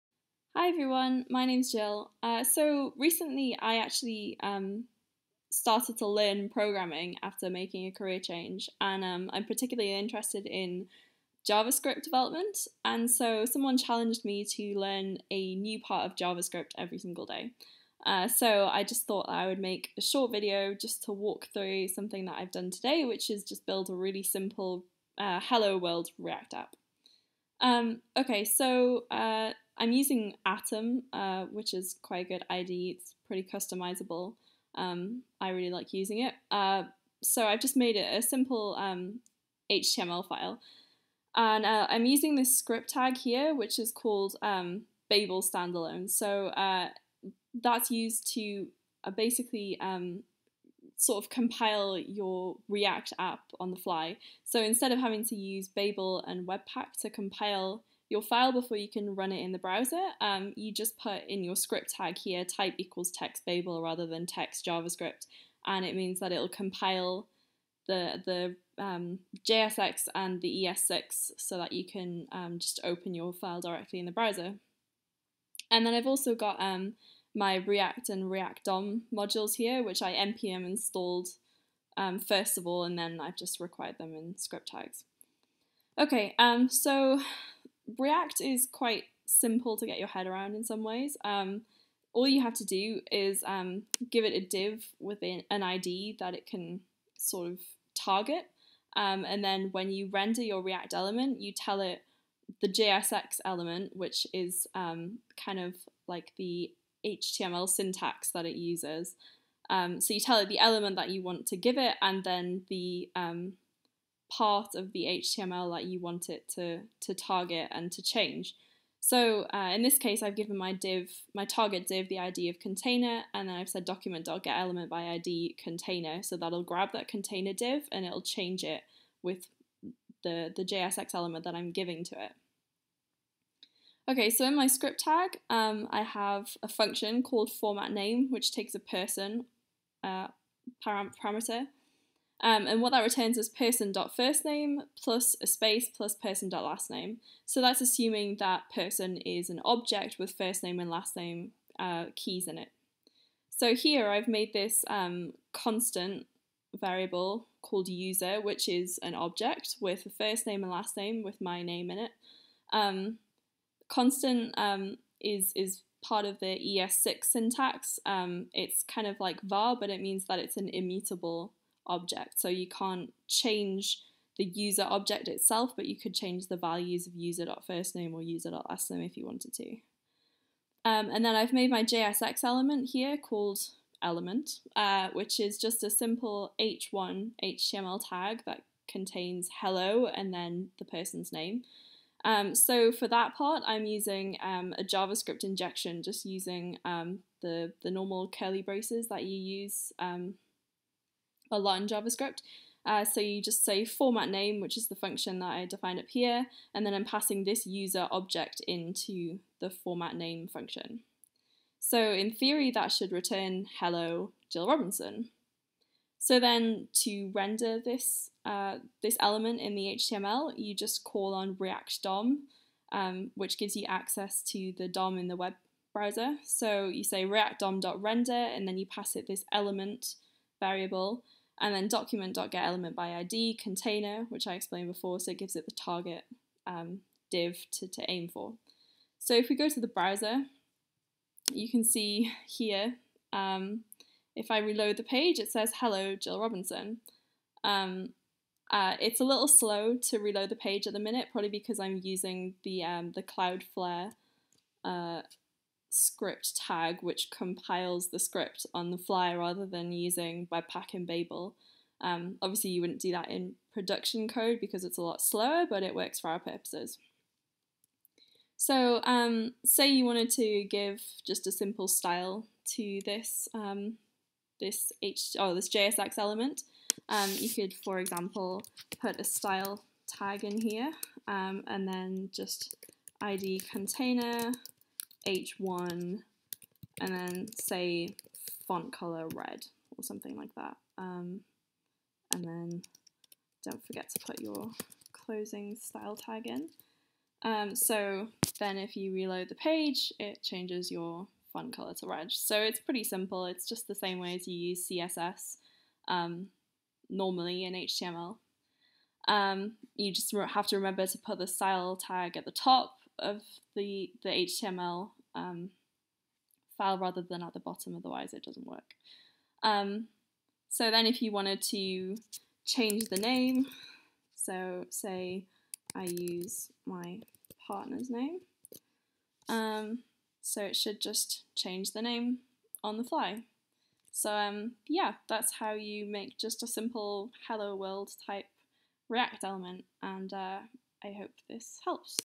JavaScript with Jill Part 1: React walkthrough
I've been challenged to learn a new part of JavaScript and record a video every day - this is a walkthrough video of a simple React 'Hello World' app that I built today!